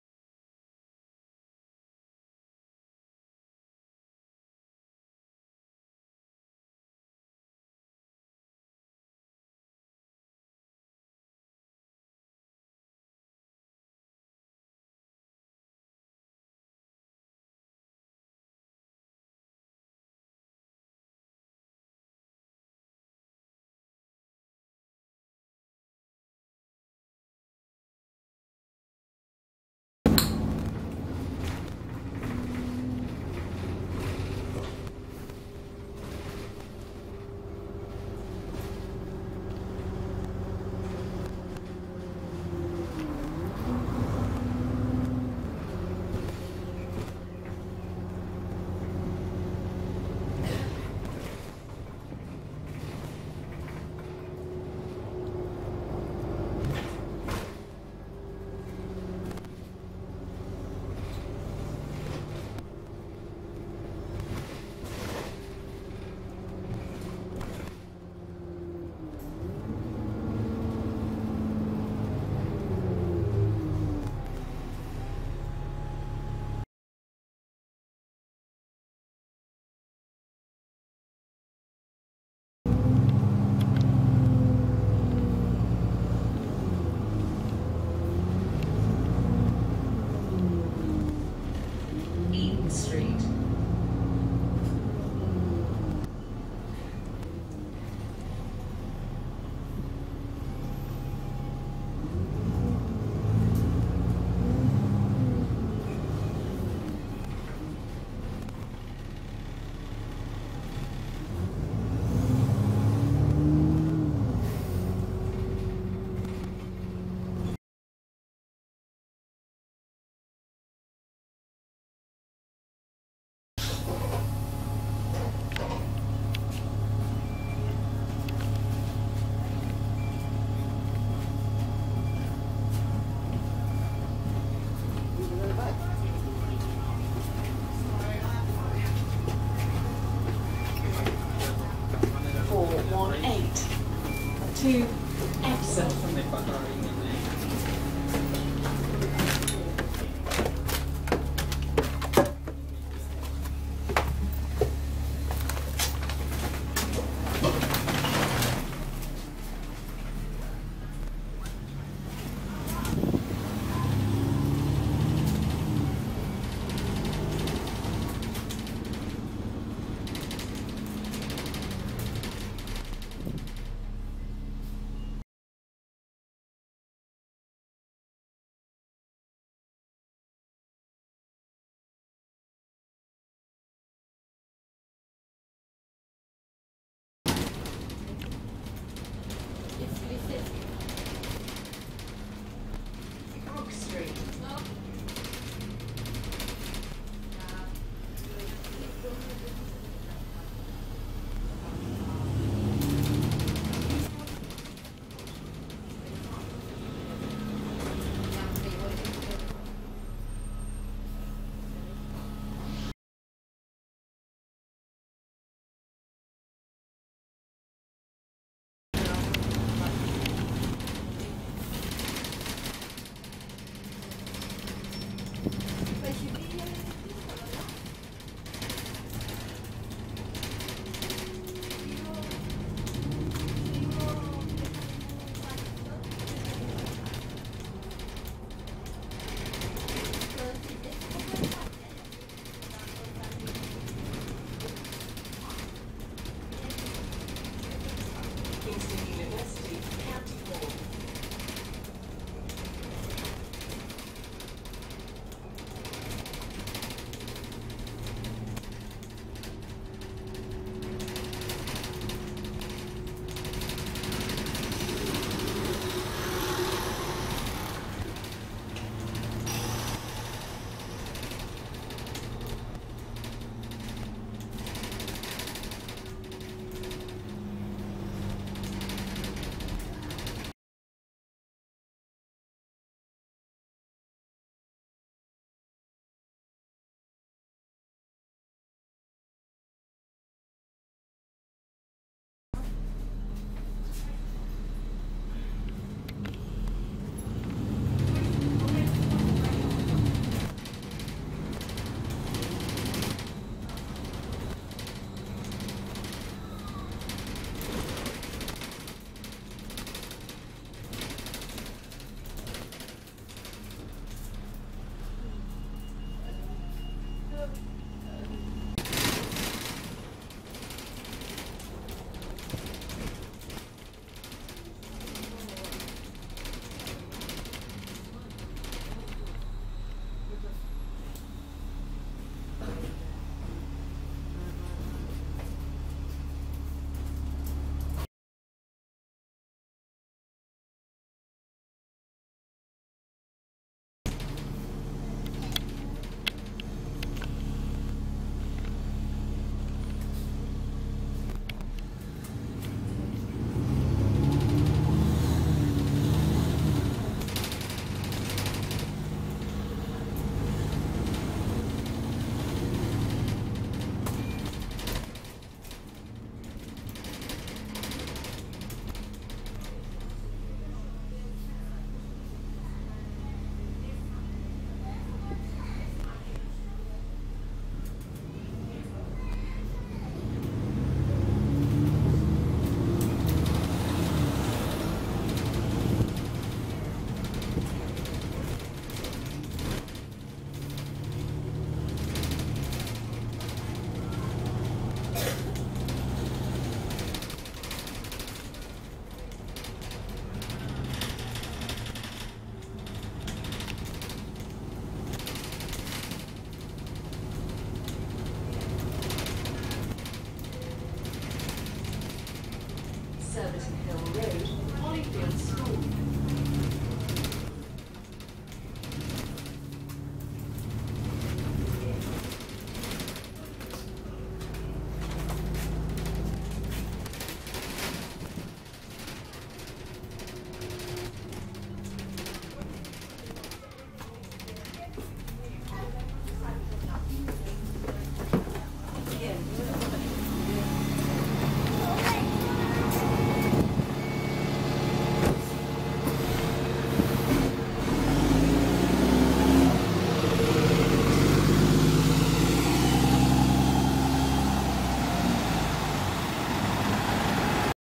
Enviro 400 Quality Line RATP (formerly DD11) ADE40303 SN11BVG on Route 418 *Short Ride*
Route 418 from Cromwell Road Bus Station to Surbiton Hill Road/Hollyfield School.